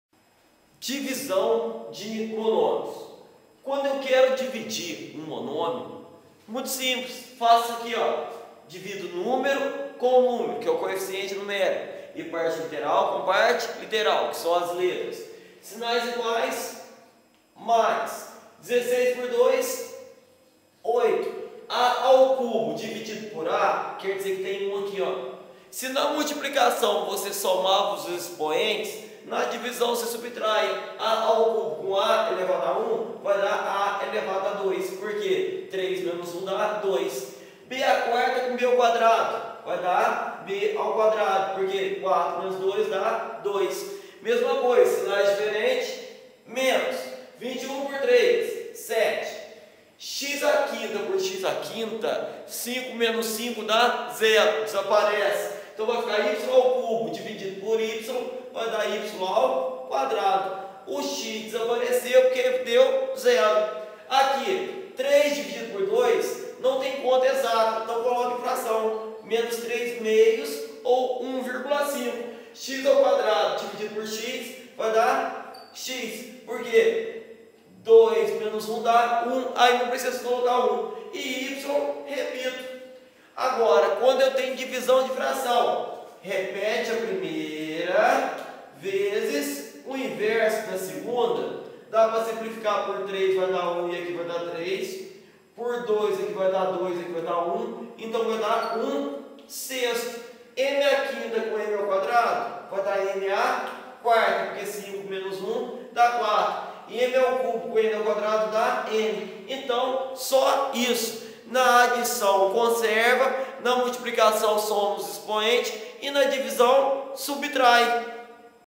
divisão de monômios com ph
Trabalhar divisão de monômios ou termo algébrico com alunos do oitavo ano do Ensino Fundamental.